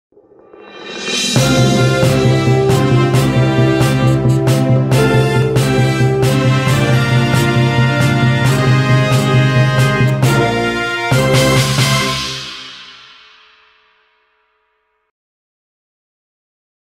Iraqi Sweets Animation - Cinema 4D
تصميم ثلاثي الابعاد للكليجة العراقية مع بعض الاكسسوارت